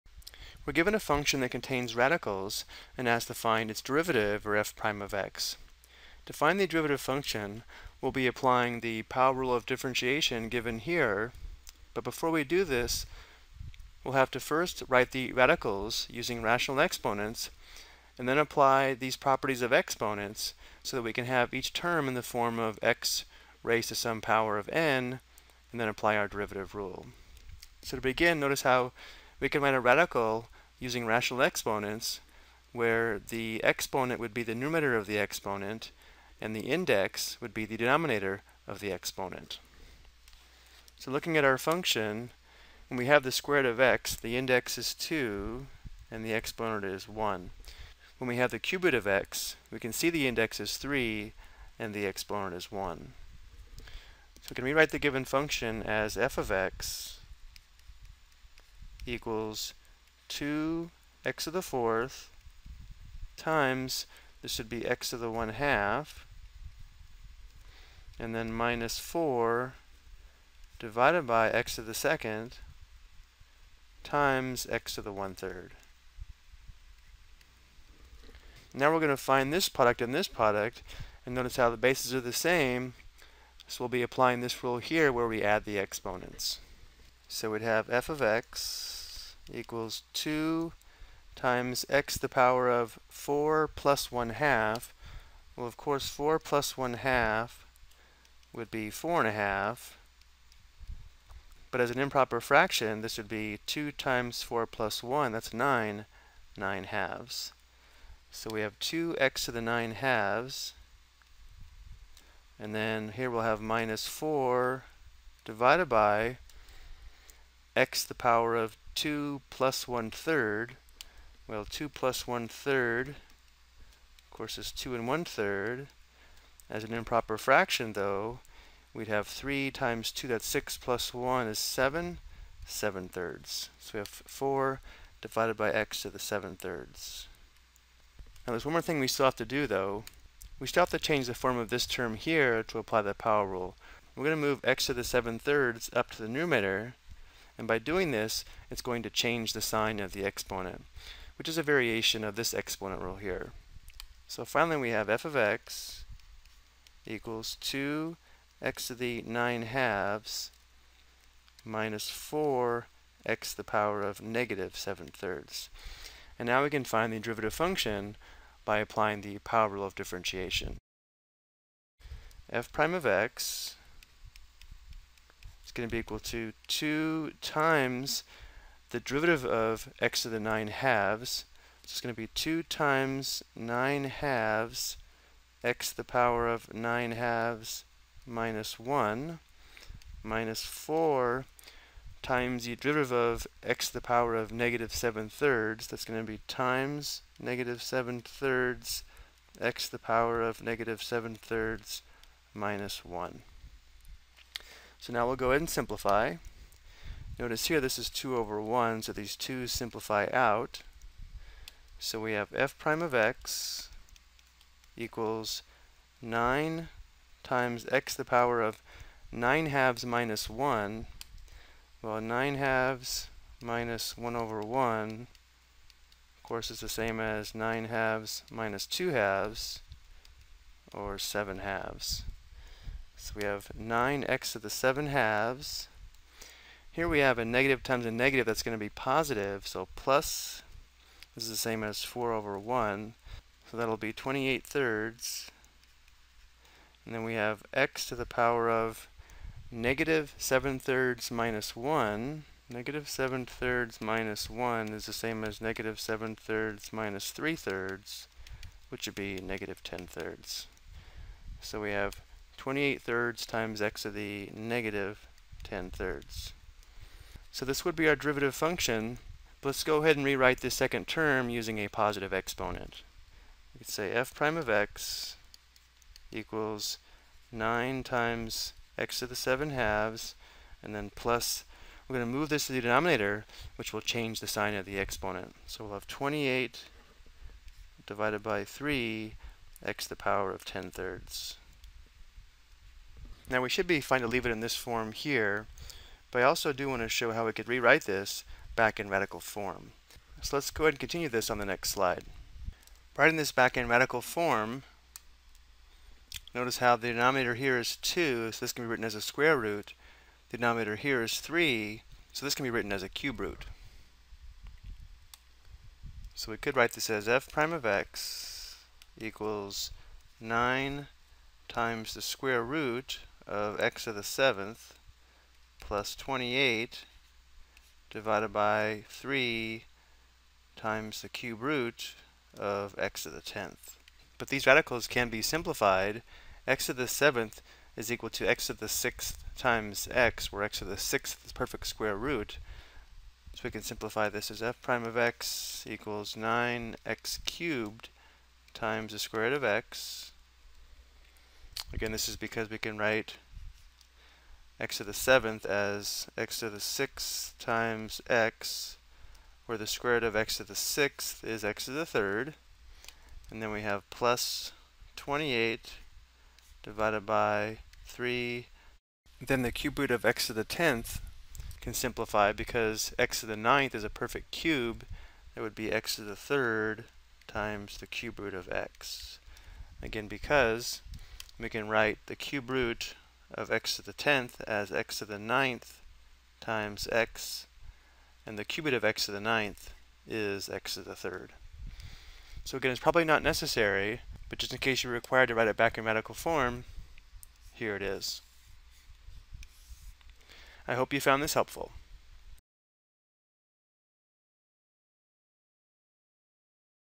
Ex: Find the Derivative of a Function Containing Radicals
This video explains how to find the derivative of a function containing radicals.